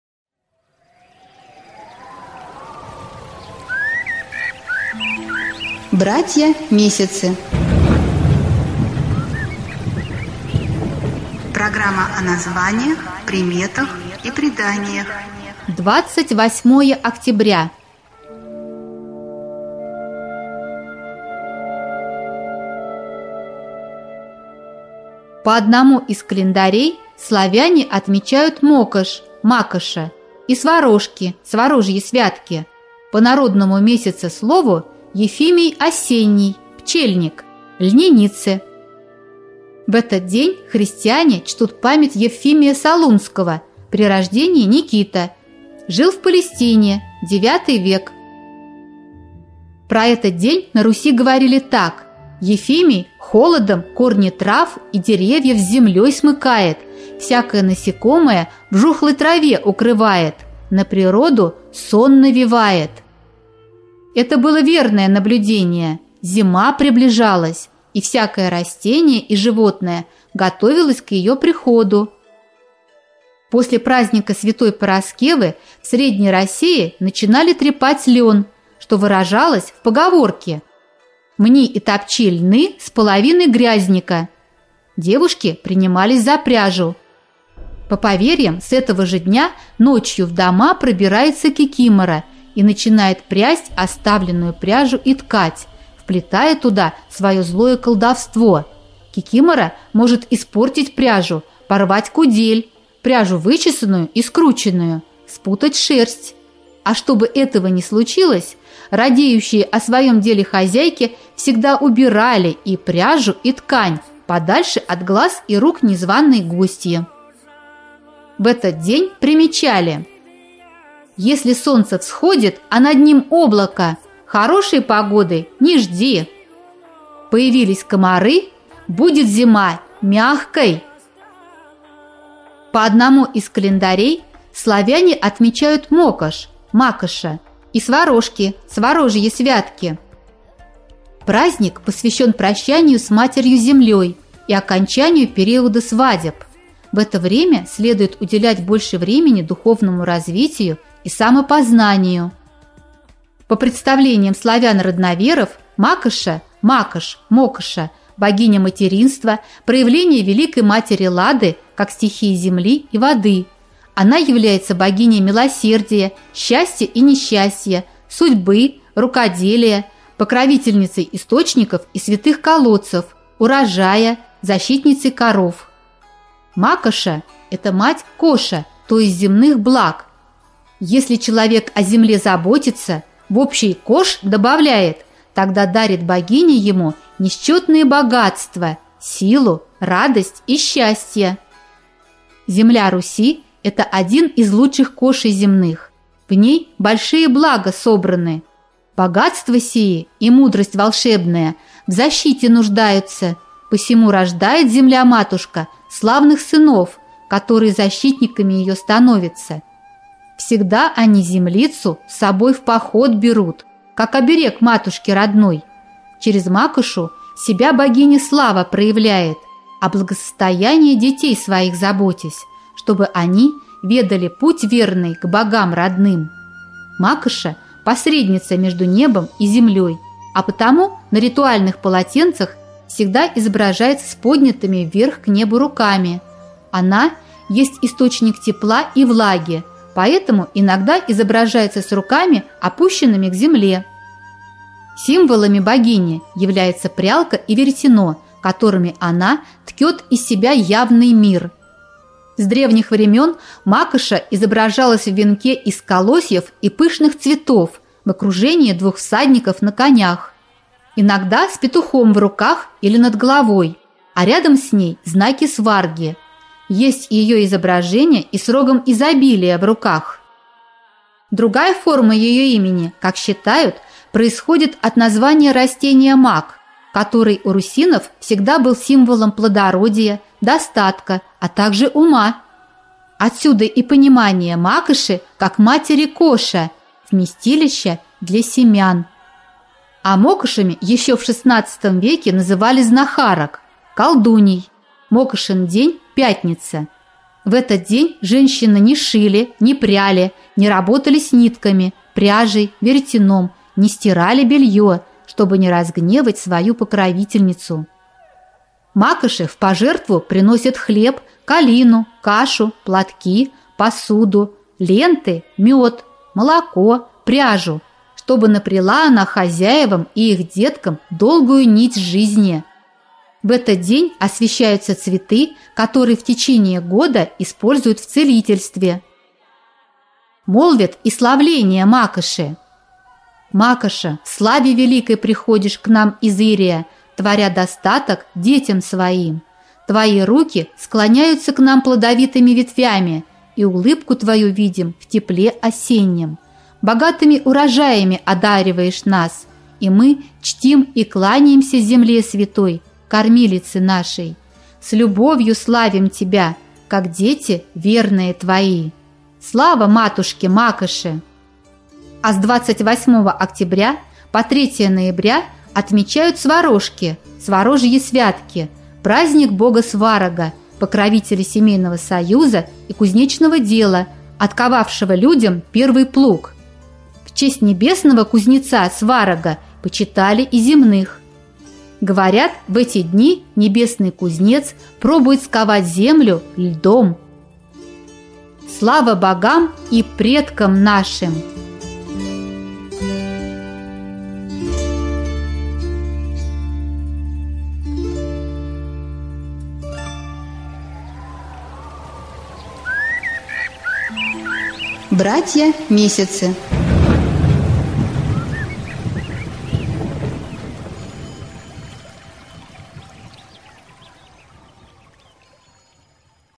28-е октября.Братья месяцы. Покров — Заимник, Свадебник.
По народному месяцеслову: Покров — Заимник, Свадебник.

Праздник Покрова Пресвятой Богородицы — один из самых почитаемых на святой Руси.

В 910 году, 14 октября (по новому стилю) в Константинополе во Влахернской церкви во время всенощного бдения одному из присутствующих в ней привиделась идущая по воздуху Царица Небесная. Преклонив колена, Богоматерь начала молиться, а потом, сняв с головы покров, распростерла его над находящимися в храме людьми, как бы укрывая или защищая их от «врагов видимых и невидимых», которые осадили Константинополь и уже готовы были праздновать победу. Явление Богоматери было воспринято молящимися как счастливое знамение.

Да и в народном календаре Покров — дата знаменательная. Не раз удивлял в этот день людей октябрь — грязник обилием выпавшего снега и крепким морозцем. Бывало, еще.

Продолжение слушайте в записи.